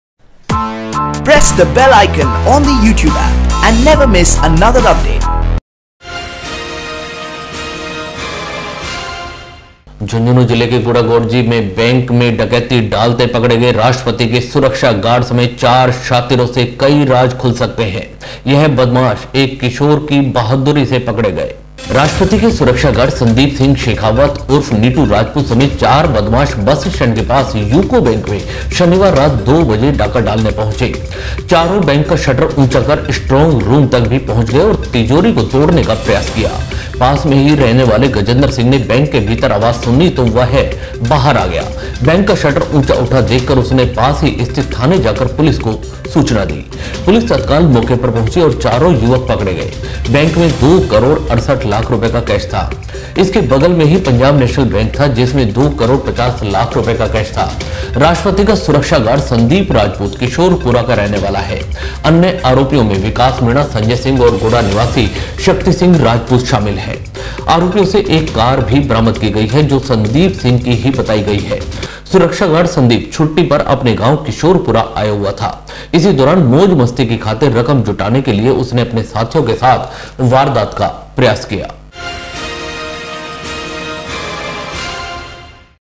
झुंझुनू - राष्ट्रपति के सुरक्षा गार्ड ने डाली बैंक में डकैती, साथियों सहित चढ़ा पुलिस के हत्थे
झुंझुनू जिले के गुढ़ागौड़जी में बैंक में डकैती डालते पकड़े गए राष्ट्रपति के सुरक्षा गार्ड समेत 4 शातिरों से कई राज खुल सकते हैं। यह बदमाश एक किशोर की बहादुरी से पकड़े गए।
 राष्ट्रपति के सुरक्षा गार्ड संदीप सिंह शेखावत उर्फ नीटू राजपूत समेत 4 बदमाश बस स्टैंड के पास यूको बैंक में शनिवार रात दो बजे डाका डालने पहुंचे थे। चारों बैंक का शटर ऊंचा कर स्ट्रांग रूम तक पहुंच गए और तिजोरी को तोड़ने का प्रयास किया। पास ही में रहने वाले गजेंद्र सिंह ने बैंक के भीतर आवाज सुनी तो वह बाहर आया। बैंक का शटर ऊंचा उठा देखकर उसने पास ही स्थित थाने जाकर पुलिस को बताया। पुलिस तत्काल मौके पहुंची और चारों युवक पकड़े गए। बैंक में 2 करोड़ 68 लाख रुपए का कैश था। इसके बगल में ही पंजाब नेशनल बैंक भी है। जिसमें 2 करोड़ 50 लाख रुपए का कैश था। राष्ट्रपति का सुरक्षा गार्ड संदीप राजपूत किशोरपुरा का रहने वाला है। अन्य आरोपियों में विकास मीणा, संजय सिंह और गुड़ा निवासी शक्तिसिंह राजपूत शामिल हैं। आरोपियों से एक कार बरामद की गई है, जो संदीप सिंह की ही बताई जा रही है। सुरक्षा गार्ड संदीप छुट्‌टी पर अपने गांव किशोरपुरा आया हुआ था। इसी दौरान मौज मस्ती की खातिर रकम जुटाने के लिए उसने अपने साथियों के साथ वारदात का प्रयास किया।